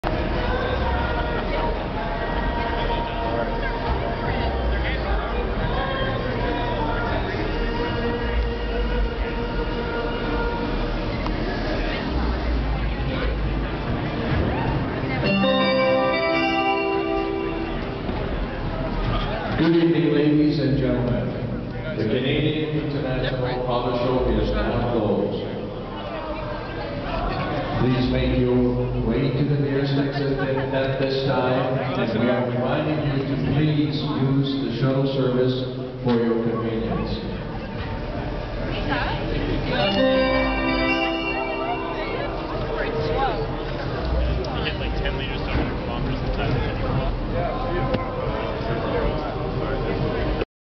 2007 Chevrolet Volt Concept at CIAS '08
The 2007 Chevrolet Volt Concept on display at the Canadian International Auto Show in Toronto. Recorded February 23, 2008.